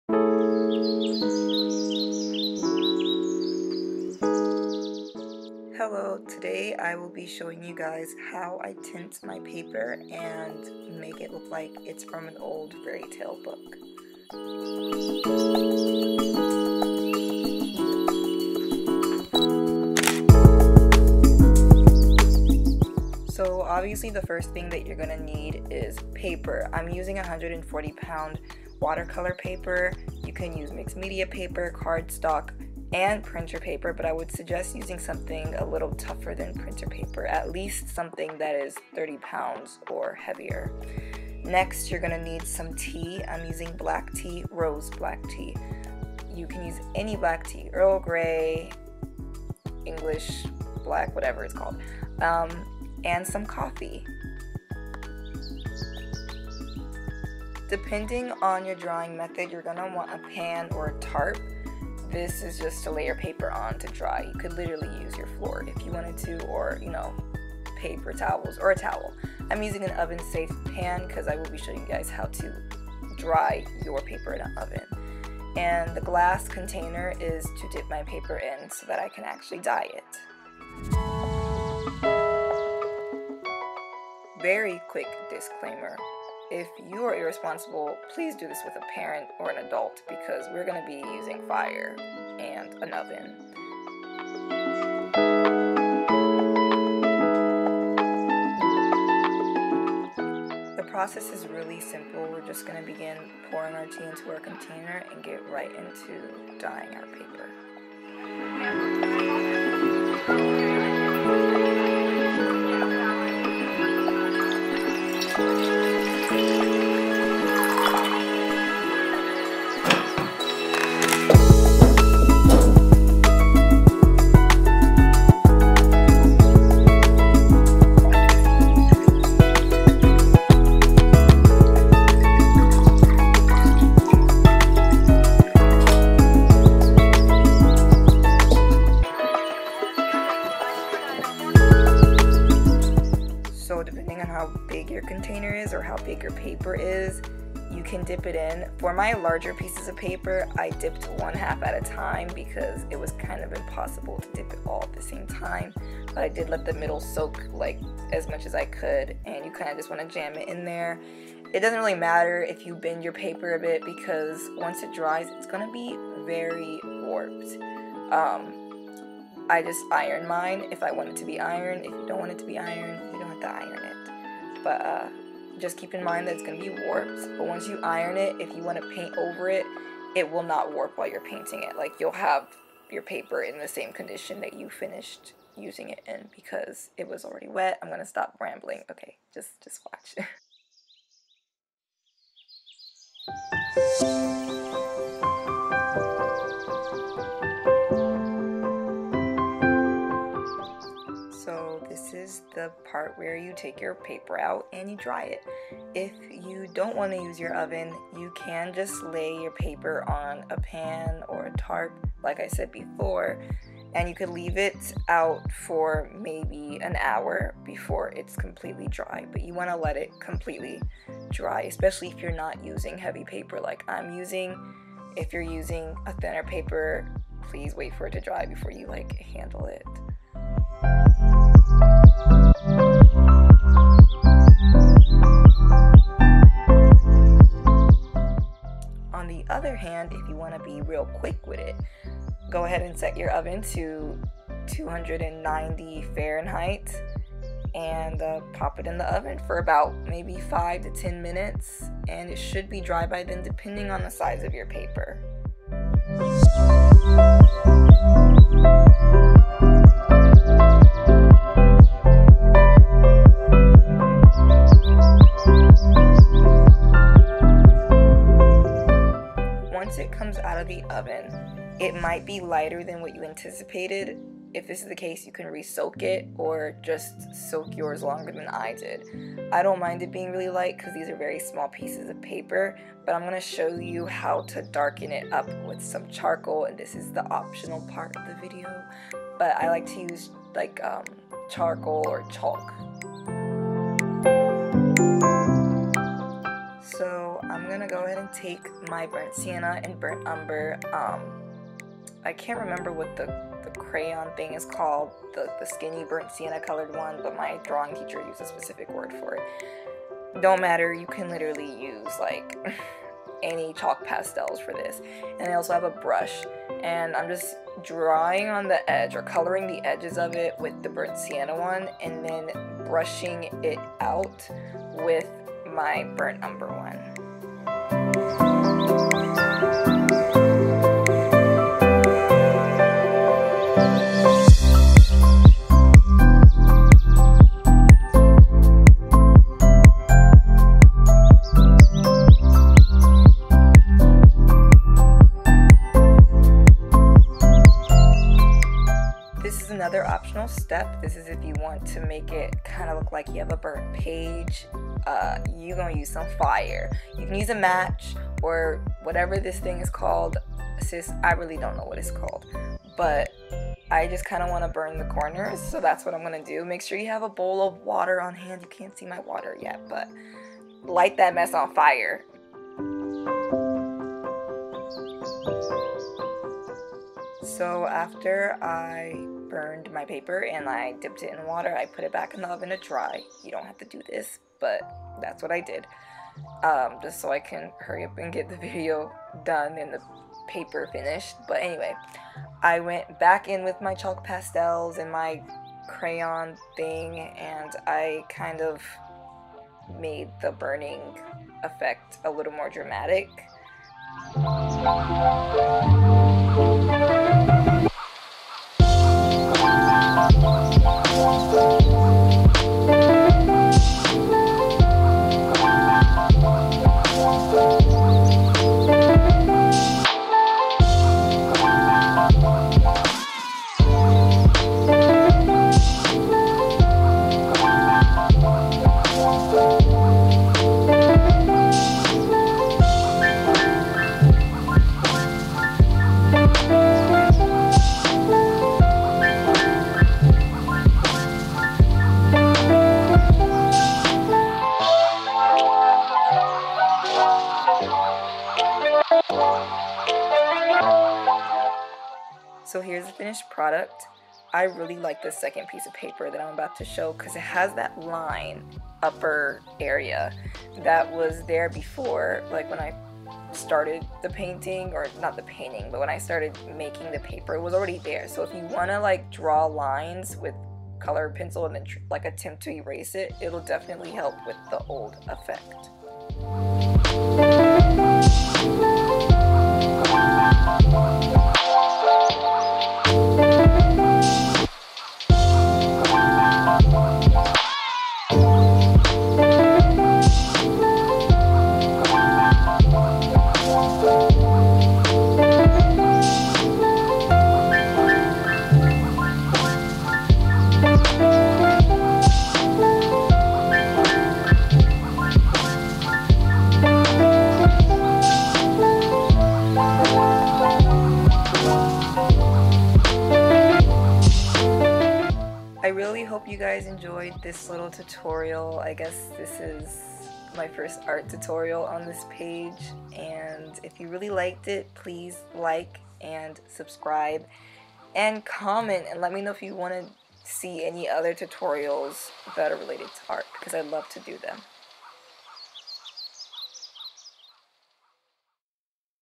old fairytale paper diy *_*
learn how to tint paper with tea/coffee and give your paper magical look! perfect for a sketch book to add variety to your work!